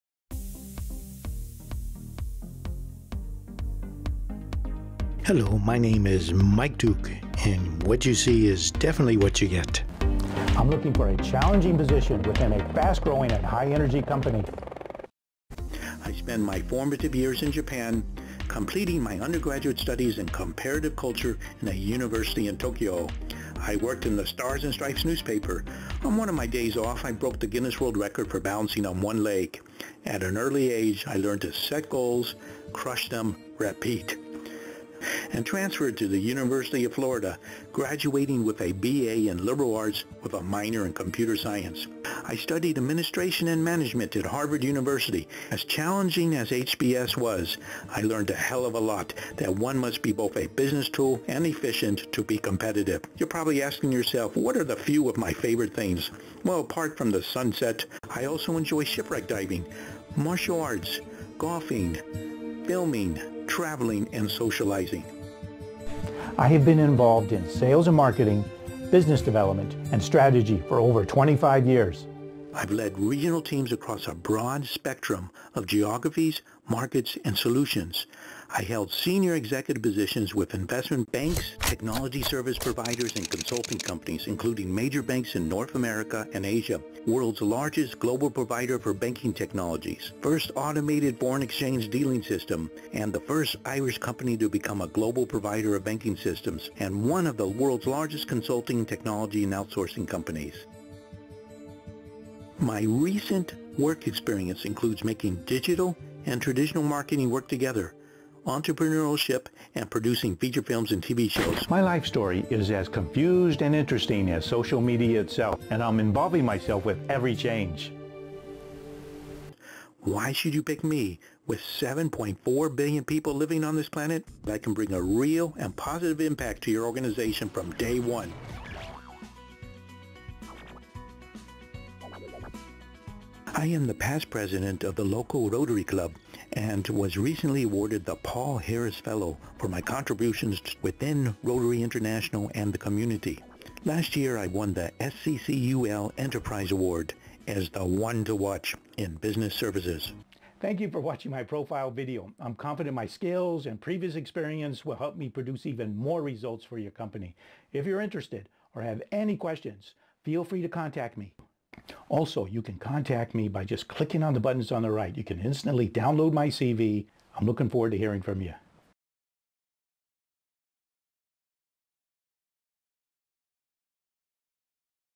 Professional Video Resume
for Michael Duke's online CV. I created a Professional Video Resume using my video editing knowledge and skills.

I am a Business Development guru, scholar of Global Affairs, Raconteur, Digital Marketing professional, and Independent TV Producer & Film Maker.

I am a Sales and Marketing Professional currently looking for a suitable job opportunity. Thank you for sharing three minutes of your time with my video CV.

I'm based in Ireland and willing to relocate for the right job opportunity.

This Video Resume is a short introduction to my qualifications, and personal characteristics. This video is meant as a supplement to my written application and my digital CV.

Please, share the video - it would mean a lot! Thank you! :)

Show support for Michael Duke on his Facebook page!

I’m looking for a Sales & Marketing position within a fast growing and high energy company.

 I spent my formative years in Japan completing my undergraduate studies in comparative culture in a University in Tokyo.

I worked in the Pacific Stars & Stripes Newspaper.  On one of my days off, I broke the Guinness World Record for balancing on one leg.  At an early age, I learned to set goals, crush them, repeat.
I attended UCLA’s Special Program in International Relations and transferred to the University of Florida, graduating with a BA in Liberal Arts with a Minor in Computer Science.

I studied administration and management at Harvard.  As challenging as Harvard Business School was, I learned a lot, that one must be both a business tool and efficient to be competitive. You are probably asking yourself what are the few of my favourite things, well apart from the sunset,  I also enjoy ship wrecking diving, martial arts, golfing, filming, travelling, and socialising.

I have been involved in sales & marketing, business development and strategy for over 25 years.  I’ve led regional teams across a broad spectrum of geographies, markets and solutions. I held senior executive positions with investment banks, technology service providers and consulting companies.

 I held senior executive positions with investment banks, technology service providers and consulting companies including major banks in North America and Asia; world’s largest global provider for banking technologies; first automated Foreign Exchange dealing system; first Irish company to become a global provider of banking systems and one of the world’s largest consulting, technology and outsourcing companies.

My recent work experience includes making digital and traditional marketing work together, entrepreneurship, and producing feature films and TV shows.

Why should you pick me with 7.4 billion people living on this planet?

As a strong communicator, who can build effective relationships. I specialise in finding creative and innovative solutions to the toughest problems. I’ve been involved in some very exciting projects. I truly believe that I can bring a real and positive impact to your organisation from day one.

I am up-to-date with Microsoft Office and have extensive experience of databases and accounting packages and digital marketing.

I am the Past President of the local Rotary Club and was awarded the Paul Harris Fellow for my contribution to Rotary International and the community. I won the SCCUL Enterprise Award as "One to Watch" in Business Services.

I’m confident my skills and experience will help me produce even more results for your company. I look forward to hearing from you.